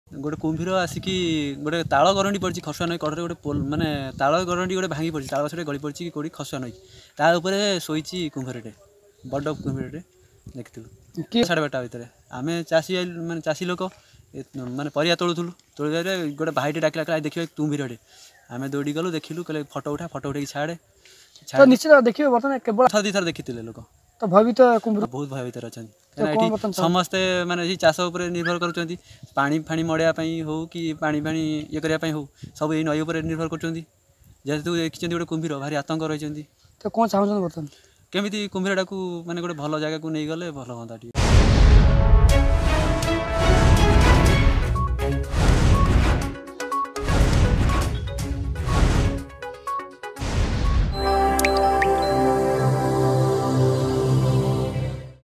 ଯାଜପୁର ଖରସ୍ରୋତା ନଦୀରେ କୁମ୍ଭୀର ଆତଙ୍କ
ଯାଜପୁର ଖରସ୍ରୋତା ନଦୀରେ କୁମ୍ଭୀର ଆତଙ୍କ... ସୂଚନା ଅନୁଯାୟୀ ଯାଜପୁର ବ୍ଲକ ସୀମିଲିଆ ପଞ୍ଚାୟତ ନଖେଇପୂର୍ ଠାରେ ଦେଖିବାକୁ ମିଳିଥିଲା । ନଦୀ କୂଳେ କୁମ୍ଭୀରଟି ଶୋଇଥିବା ଅବସ୍ଥାରେ ଦେଖି ସ୍ଥାନୀୟ ଲୋକେ ଭୟଭିତ ହୋଇପଡିଛିନ୍ତି ...
ଅଧିକ ଖବର ପାଇଁ ଡ଼ାଉନଲୋଡ କରନ୍ତୁ “Argus News” ଆପ୍